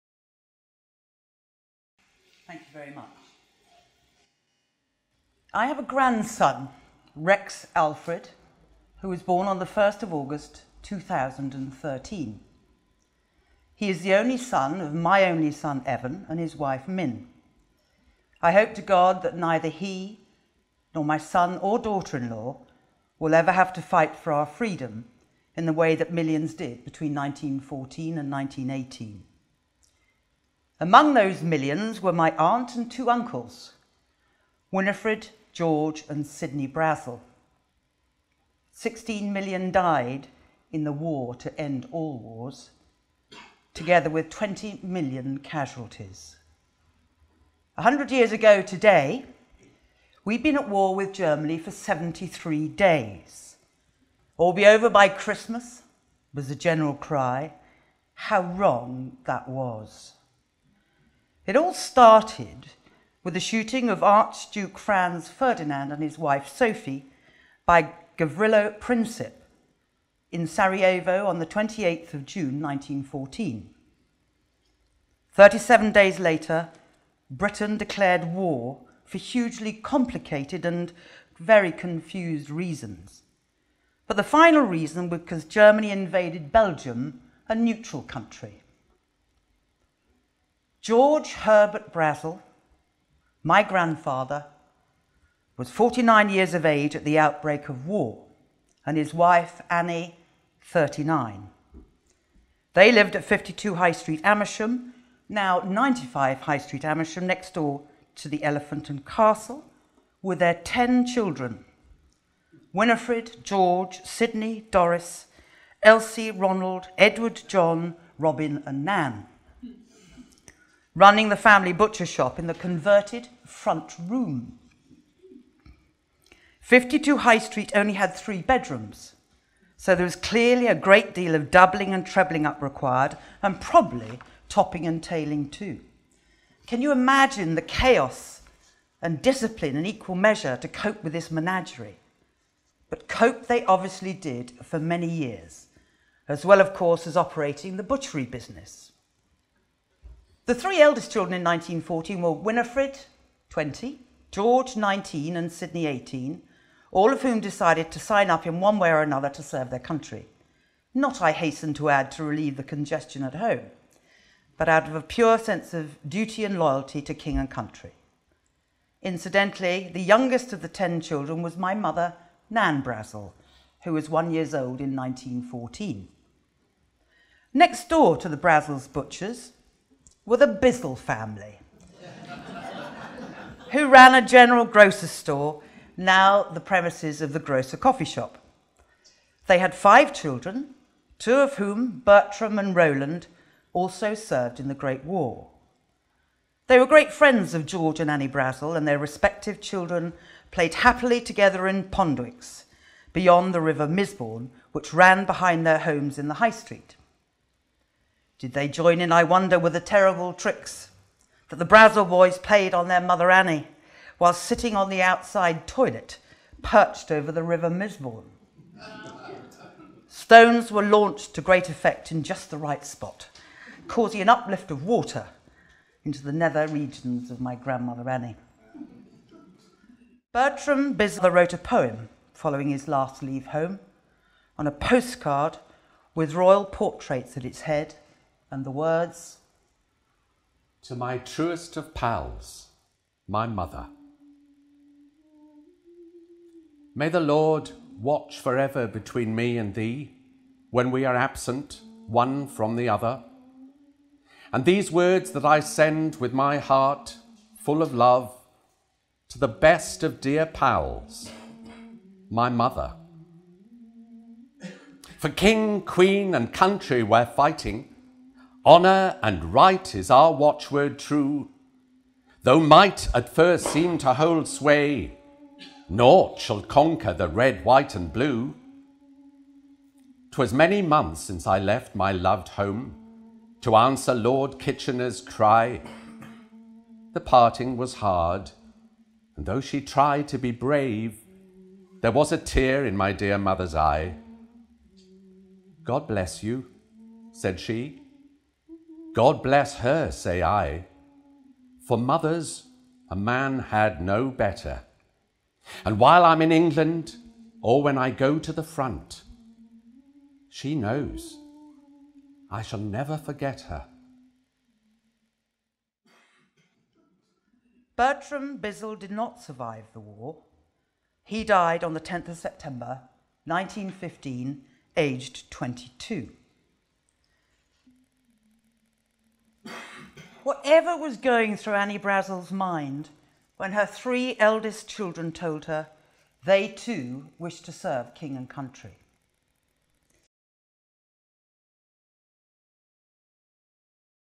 The Brazils of Amersham in The Great War - Home in Amersham
Annie Hamilton-Pike sets the scene of the family butchers in the High Street Amersham just before the war.